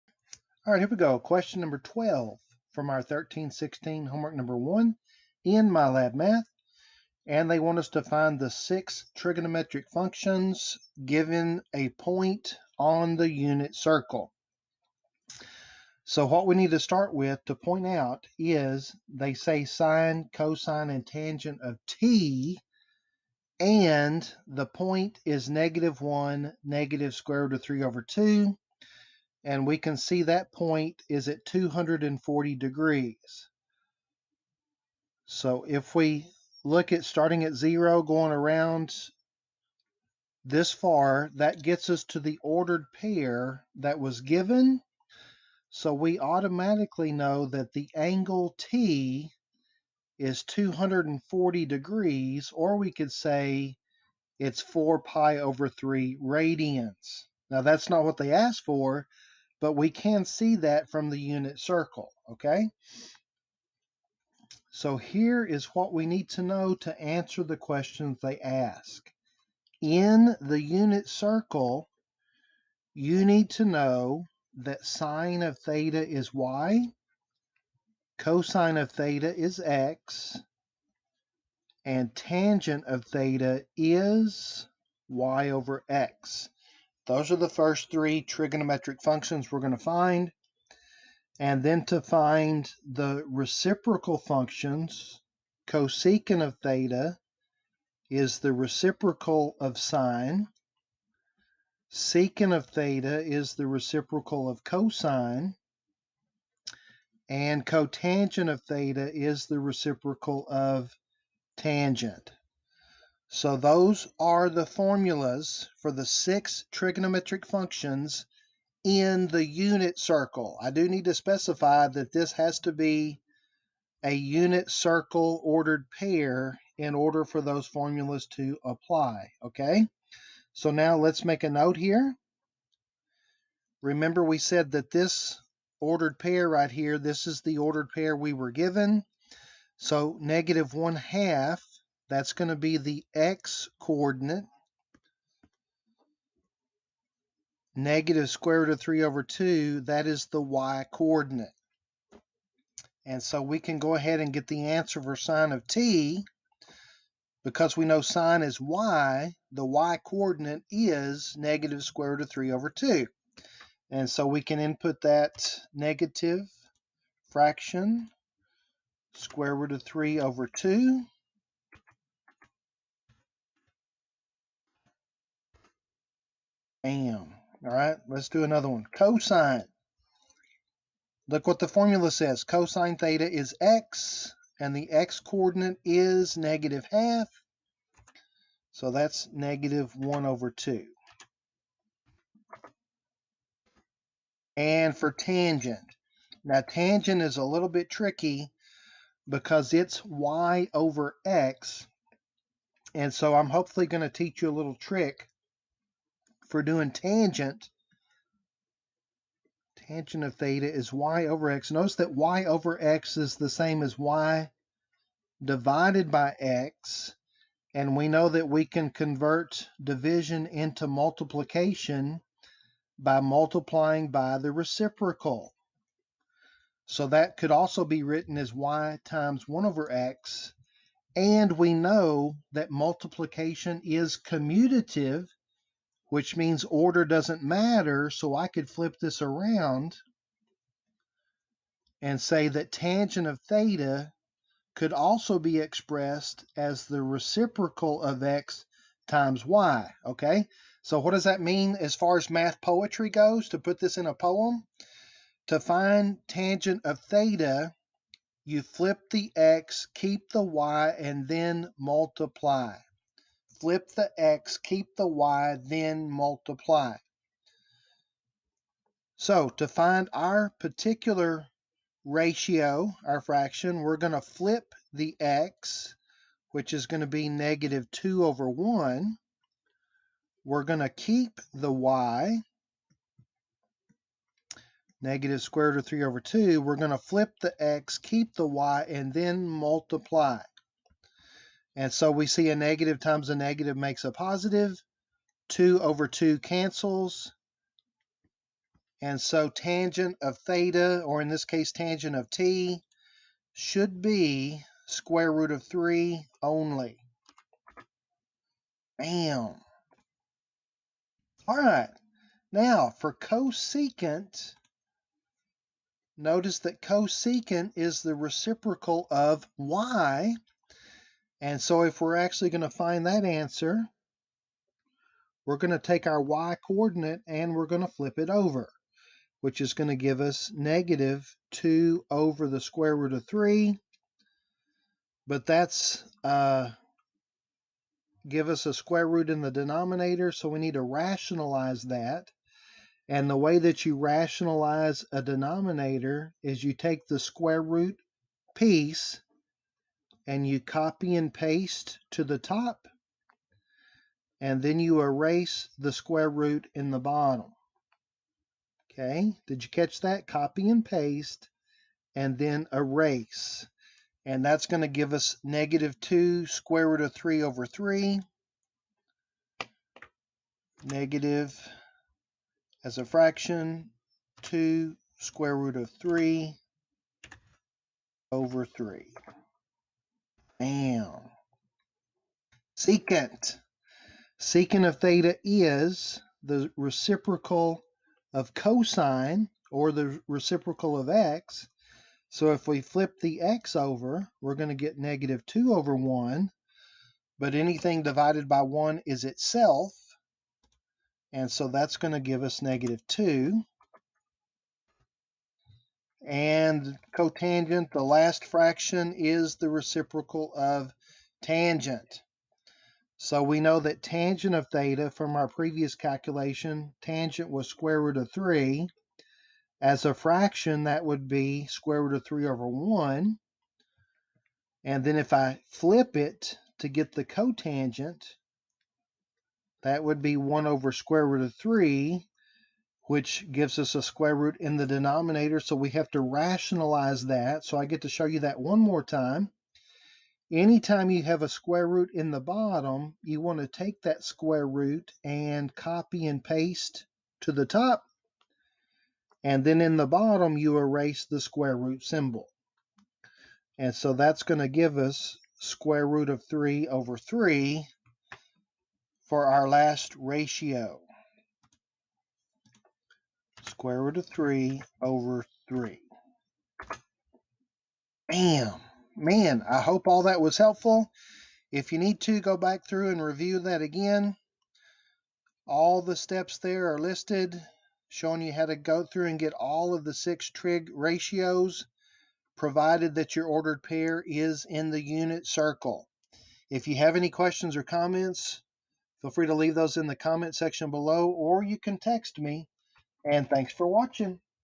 Trigonometry - HW#1 Question #12 & Precalculus - HW#3 Question #9 - MyLab Math
Finding the 6 trigonometric functions given an ordered pair in the unit circle.